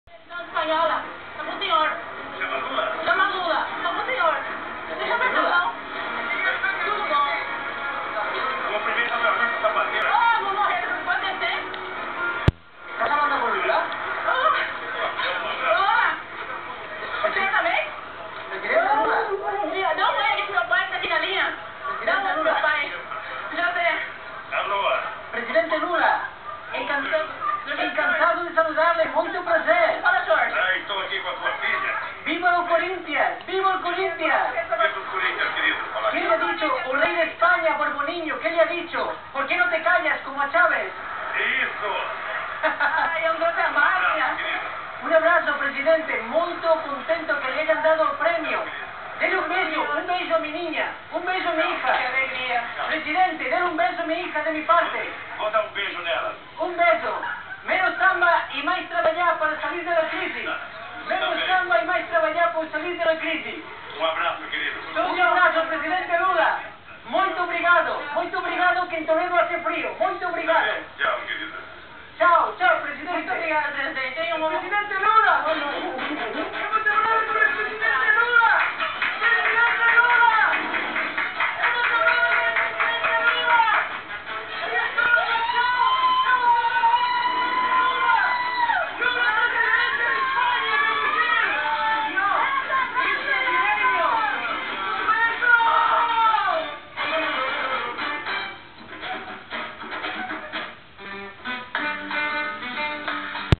Lula na TV espanhola.
Visita de Lula a Espanha. A TV nao perdeu uma chance de fazer uma chacota com o presidente.